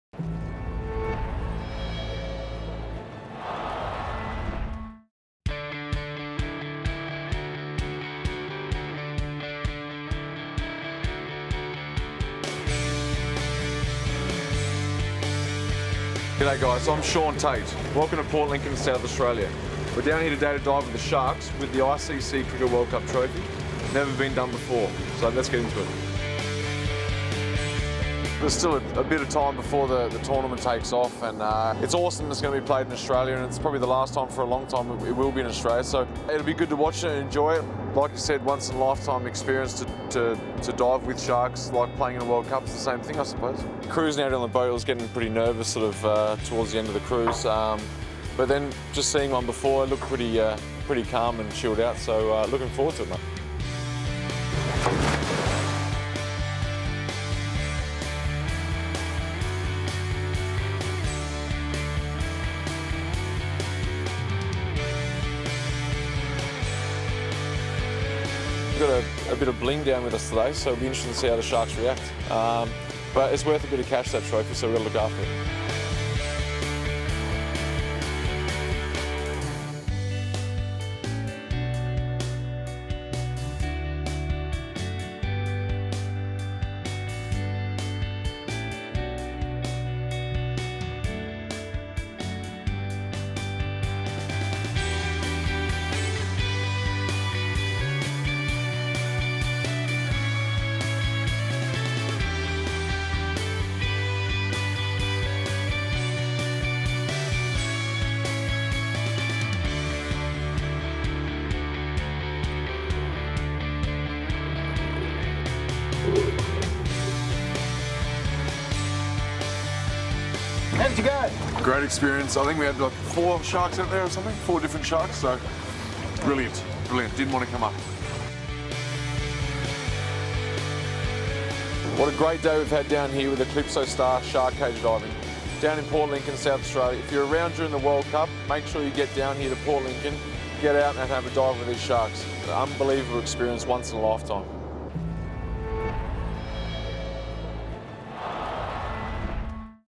Shaun Tait World Cup Shark Dive
Australian cricketer Shaun Tait took the ICC Cricket World Cup Trophy for a dip in the lead-up to the 2015 tournament. The trophy ended up deep underwater face to face with some monsters of the deep in a Port Lincoln shark dive in South Australia.